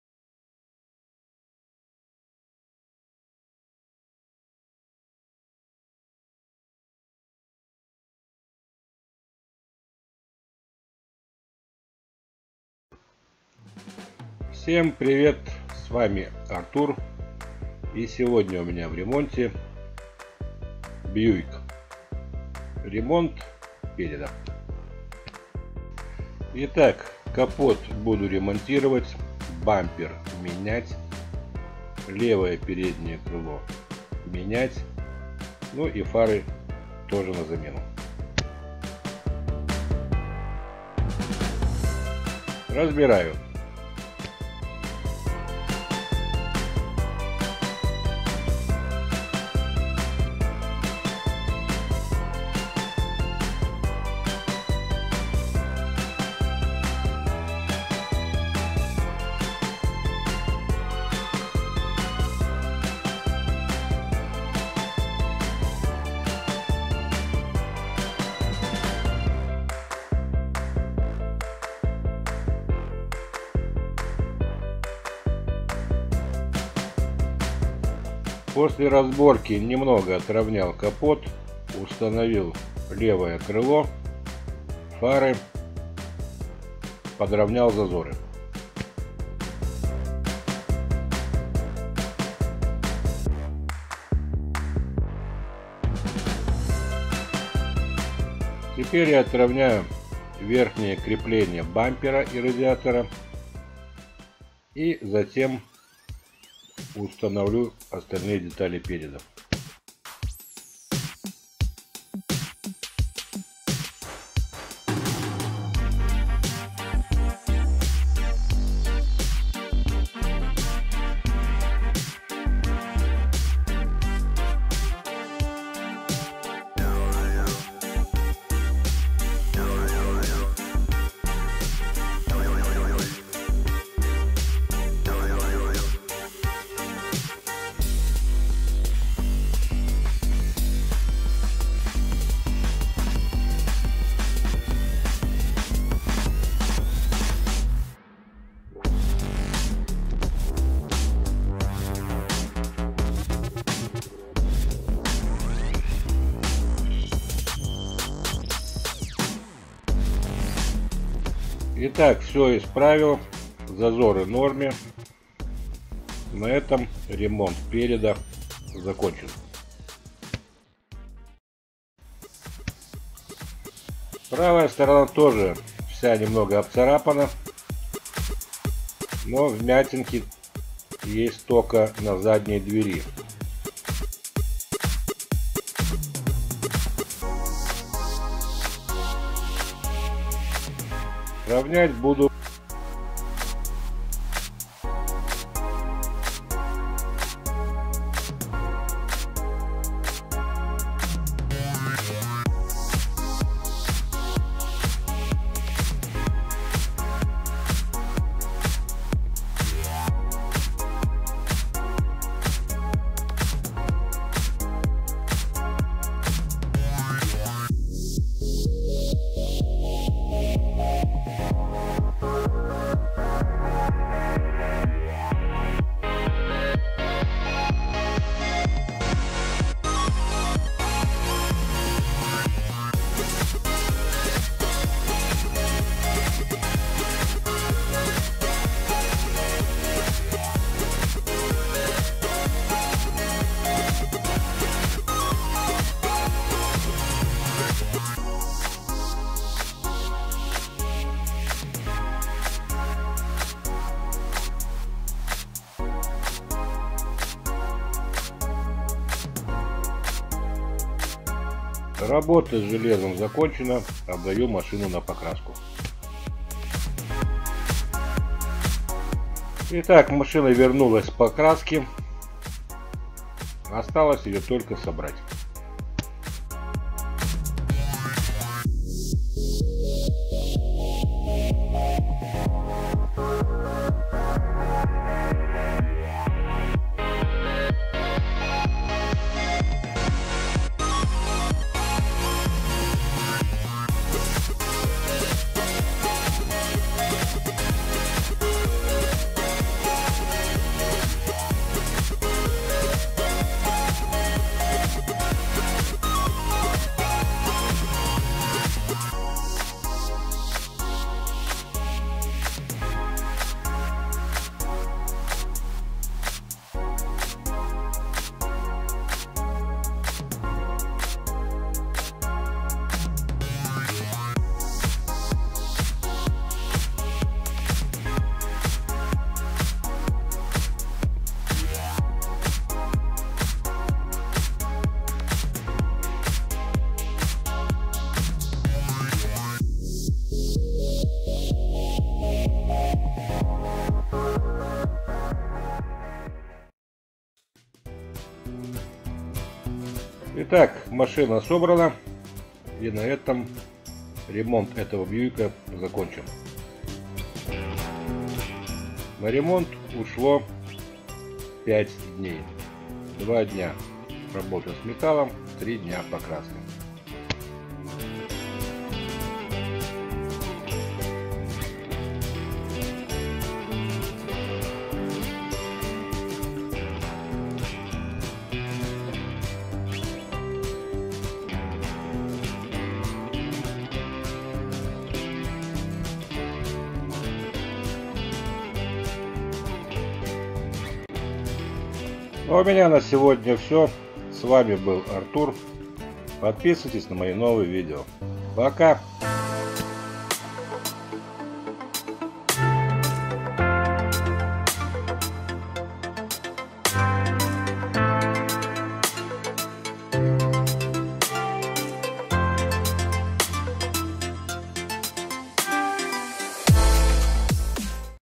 Buick Encore. The front end repair. Ремонт переда.
Body repair. The front end repair.
Ремонт кузова. Ремонт переда.